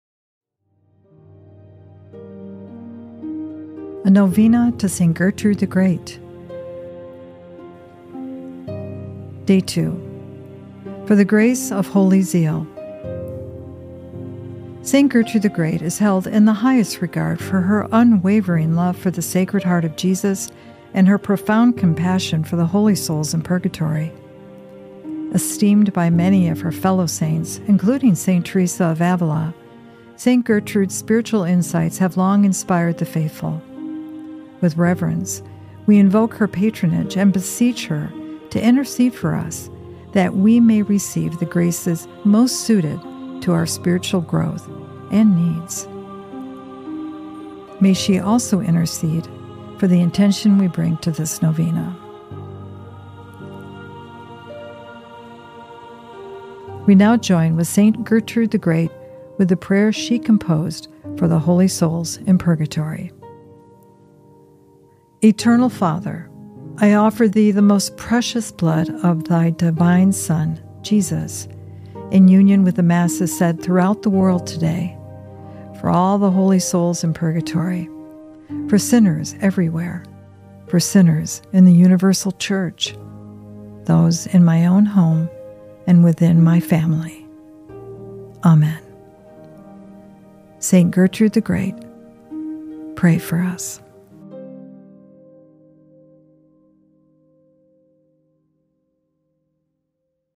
Day 2 – St. Gertrude the Great Novena – Discerning Hearts Catholic Podcast
St. Gertrude the Great Novena – Day 2 – For the Grace of Holy Zeal

St. Gertrude the Great is held in the highest regard for her unwavering love for the Sacred Heart of Jesus and her profound compassion for the Holy Souls in Purgatory. Esteemed by many of her fellow saints, including St. Teresa of Avila, St. Gertrude’s spiritual insights have long inspired the faithful. With reverence, we invoke her patronage and beseech her to intercede for us that we may receive the graces most suited to our spiritual growth and needs (mention personal intention). May she also intercede for the intention we bring to this novena.

We join St. Gertrude in a prayer she composed for the Holy Souls in Purgatory:

Eternal Father,
I offer Thee the Most Precious Blood of Thy Divine Son,
Jesus, in union with the Masses said throughout the world today,
for all the holy Souls in Purgatory,
for sinners everywhere,
for sinners in the Universal Church,
those in my own home and within my family.
Amen.

St. Gertrude the Great, pray for us